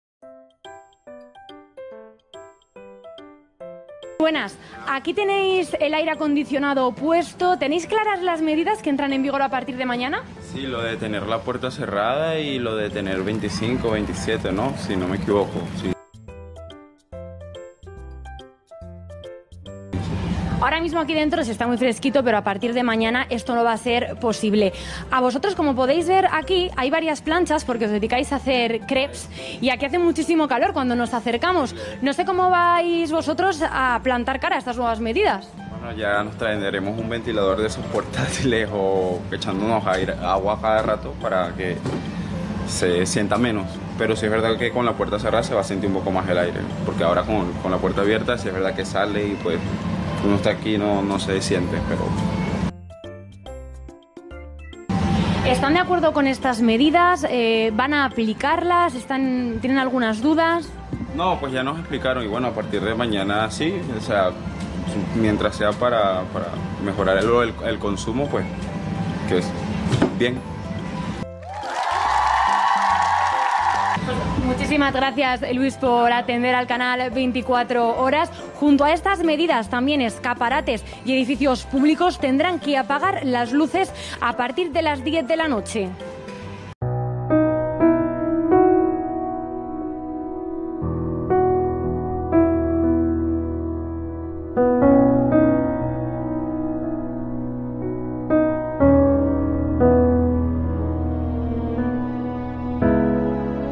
TVE vuelve a intentar manipular: no funciona 😏
Otra vez alguien de la TV entrevista a personas informadas que no tienen problema con cumplir la ley. Ya es mala suerte...